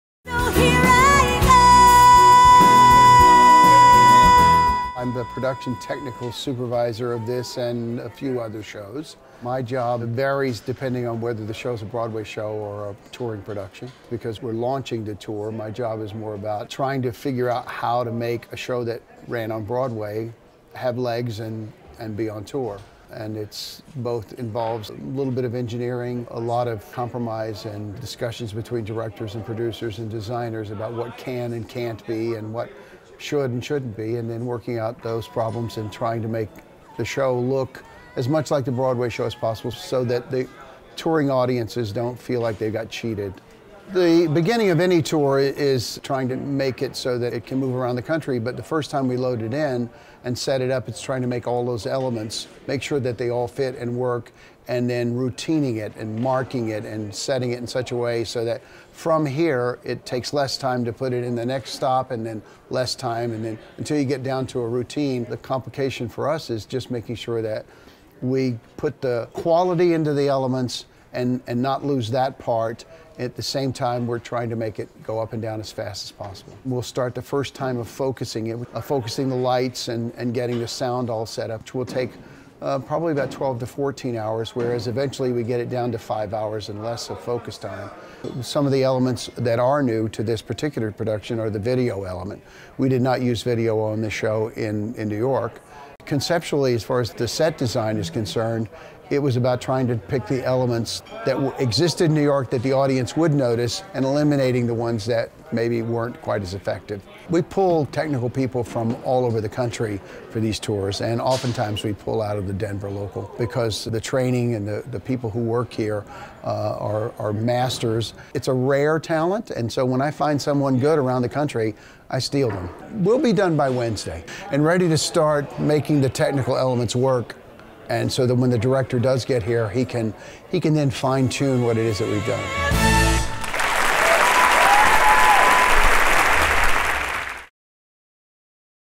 'If/Then': How the set was installed for Denver
We previously showed you our time-lapse video showing how crews over four days installed the set for the launch of the national touring production of ''If/Then' at The Buell Theatre in Denver. Here Production Technical Supervisor Jake Bell talks about the particular challenges of readying the set not only for Denver, but for theatres of various sizes throughout the country. One thing that is new to this particular production is a wall of video that is used in the show. "We did not use the video element in New York," Bell said. 'If Then,' by the creators of 'Next to Normal,' follows two distinct storylines in the life of Elizabeth, a modern woman who woman faces the intersection of choice and chance. Video by David Lenk and John Moore for the DCPA NewsCenter. Video by David Lenk and John Moore for the DCPA NewsCenter.